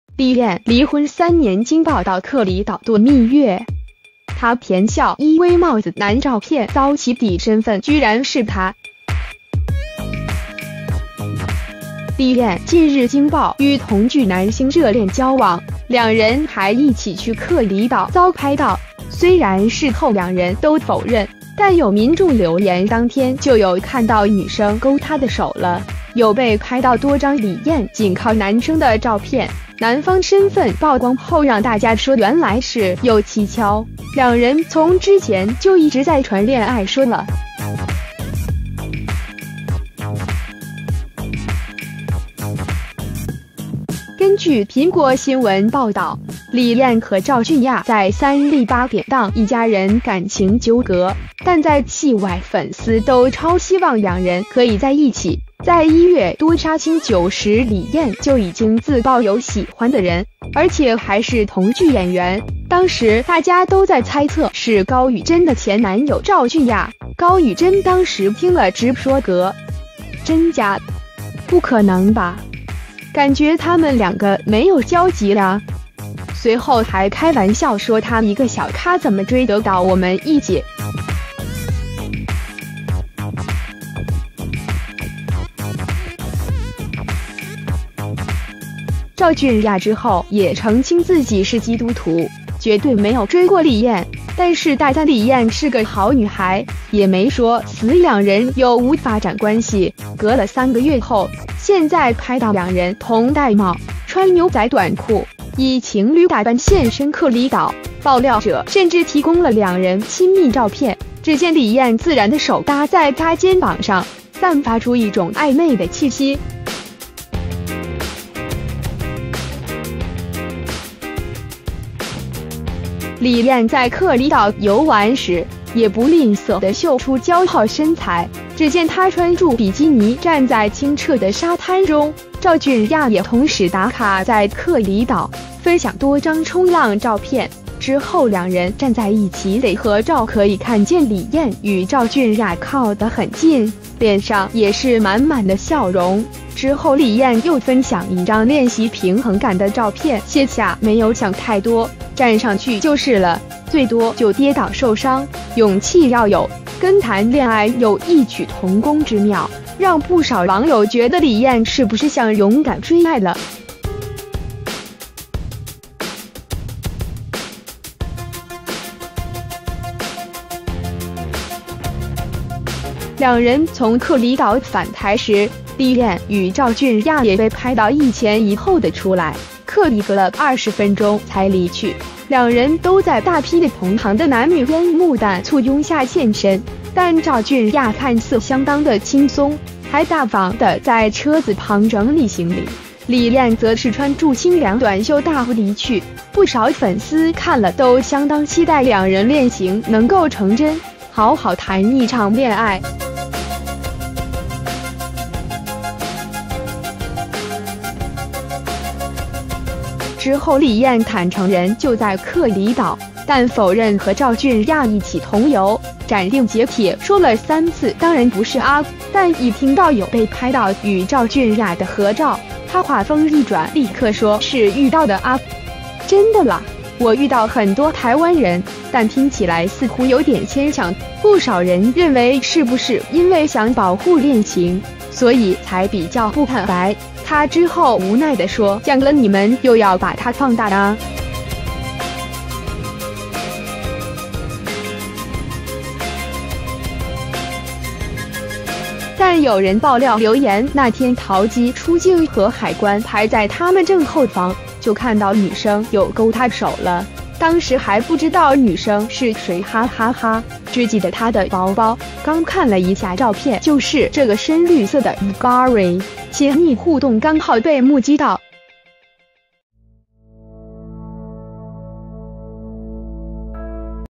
李燕離婚3年驚爆「到峇裡島度蜜月」！她甜笑「依偎帽子男」照片遭起底身分居然是他
李燕近日驚爆與同劇男星熱戀交往，兩人還一起去峇裡島遭拍到，雖然事後兩人都否認，但有民眾留言「當天就有看到女生勾他的手了~~」，有被拍到多張李燕緊靠男生的照片，男方身分曝光後讓大家說原來事有蹊蹺，兩人從之前就一直再傳戀愛說了。根據《蘋果新聞》報導，李燕和趙駿亞在三立8點檔《一家人》感情糾葛，但在戲外粉絲都超希望兩人可以在一起，在1月多殺青酒時李燕就已經自爆有喜歡的人，而且還是同劇演員，當時大家都在猜測是高宇蓁的前男友趙俊亞，高宇蓁當時聽了直說「蛤？真假？不可能吧！感覺他們兩個沒有交集呀！」隨後還開玩笑說「他一個小咖怎麼追得到我們一姐！」。

兆駿亞之後也澄清自己是基督徒，絕對沒有追過李燕，但是大讚李燕是個好女孩，也沒說死兩人有無發展關係，隔了3個月後，現在拍到兩人同戴帽、穿牛仔短褲，以情侶打扮現身峇裡島，爆料者甚至提供了兩人親密照片，隻見李燕自然的手搭在他肩膀上，散發出一種曖昧的氣息。李燕在峇裡島遊玩時，也不吝嗇得秀出姣好身材，隻見她穿著比基尼站在清澈的沙灘中，兆駿亞也同時打卡在峇裡島，分享多張衝浪照片，之後兩人站在一起得合照可以看見李燕與兆駿亞靠得很近，臉上也是滿滿的笑容，之後李燕